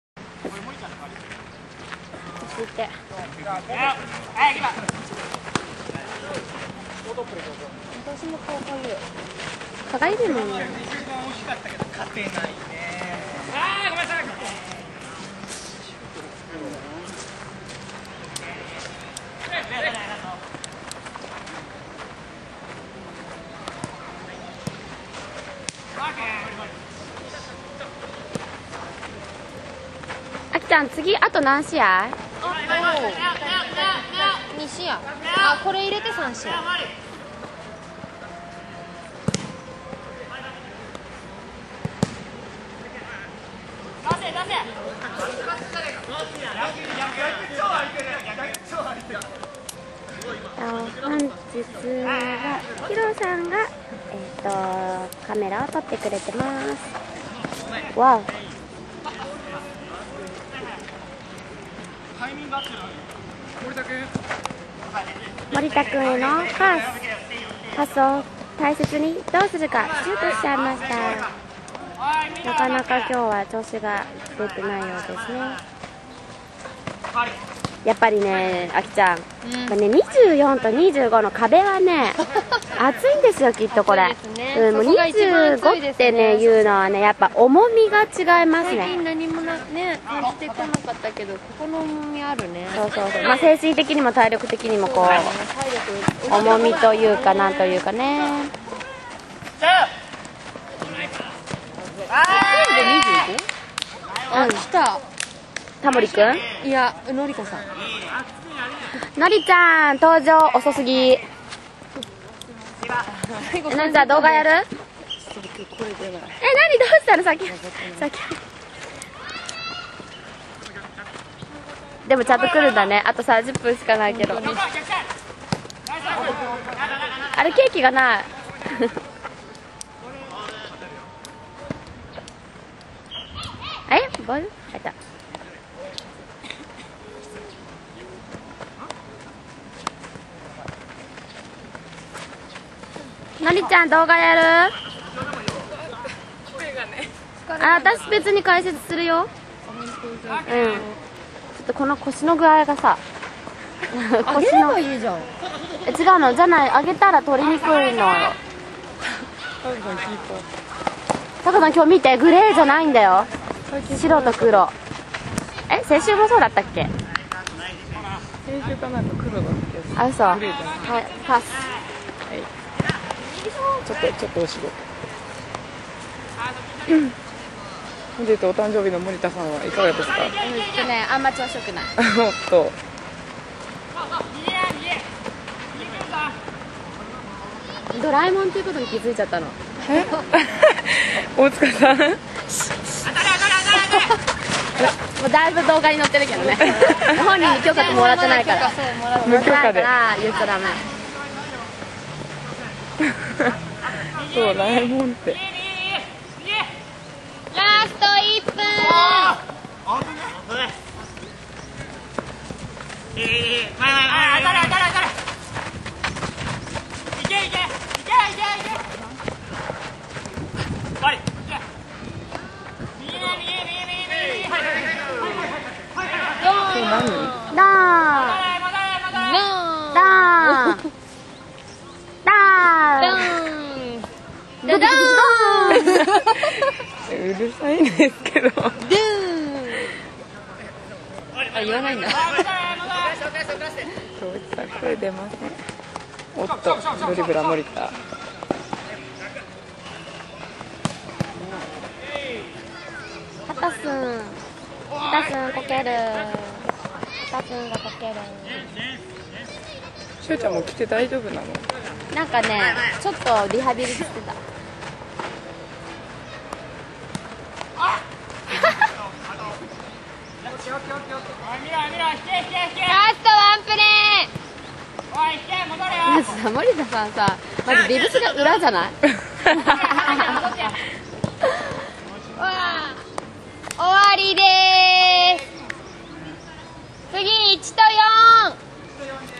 第20回アオキング 2010/06/13 リーグ戦 青VS黒
毎週日曜日の朝９：００～１１：００に都立青山公園で競技志向の個人参加型の草 フットサルを開催しています。